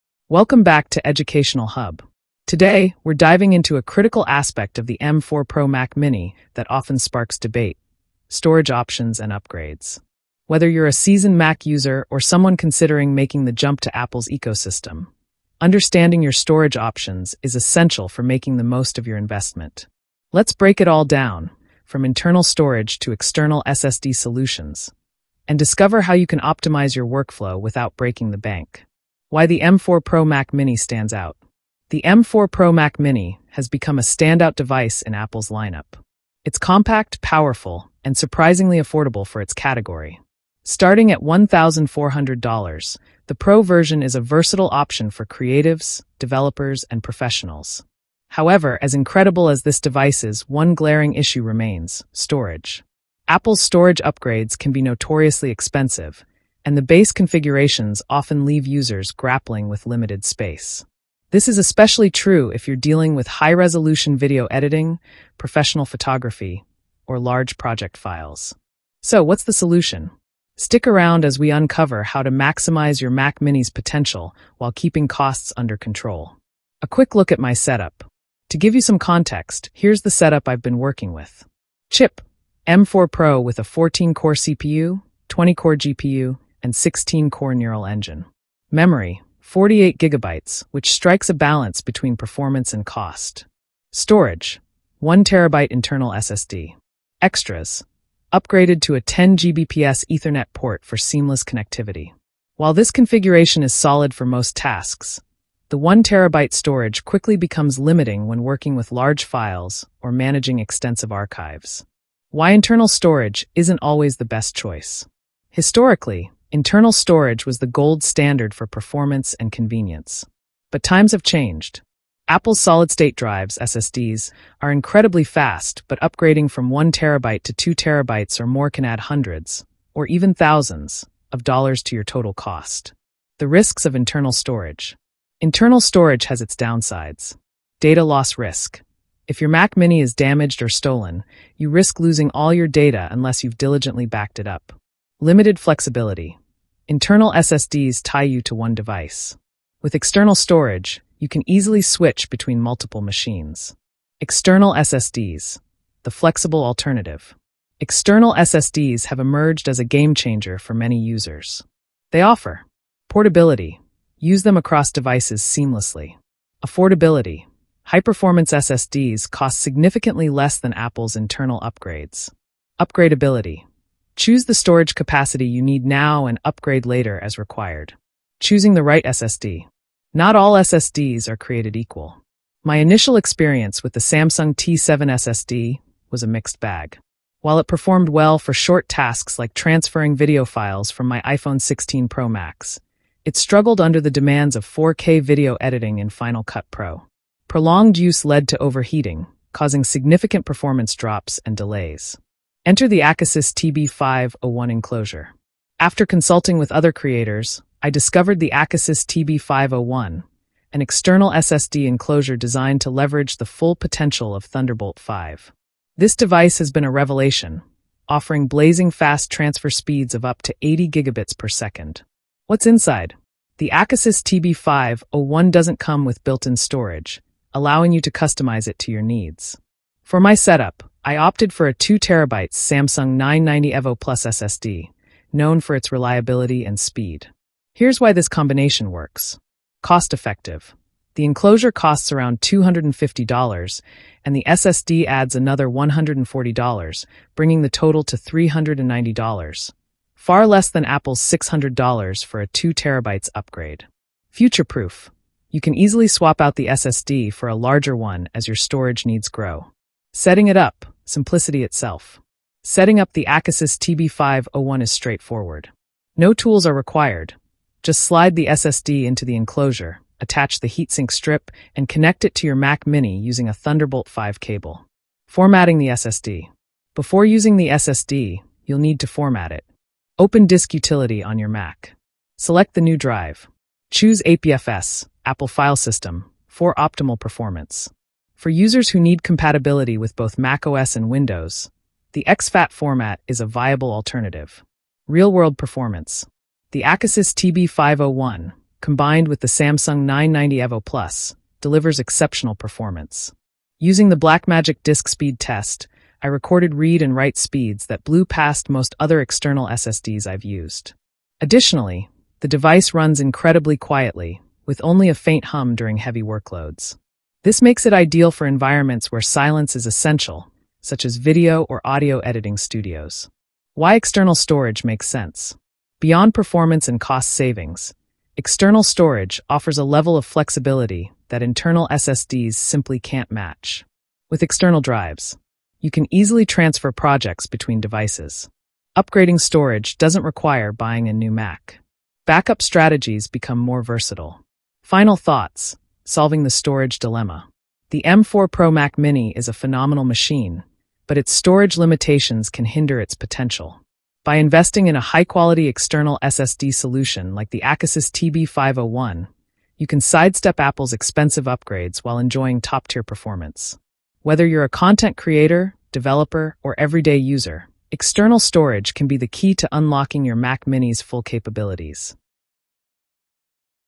M4 Pro Mac mini STORAGE SOLUTION FOUND!
Are you tired of dealing with storage issues on your M4 Pro Mac mini? Well, you're in luck because we've finally found a solution to this frustrating problem! In this video, we'll walk you through the steps to fix the storage issue on your M4 Pro Mac mini once and for all. From diagnosing the problem to implementing the solution, we'll cover it all. Whether you're a tech enthusiast or just a Mac user looking for a fix, this video is for you. So, what are you waiting for? Watch now and breathe a sigh of relief as you say goodbye to storage woes on your M4 Pro Mac mini!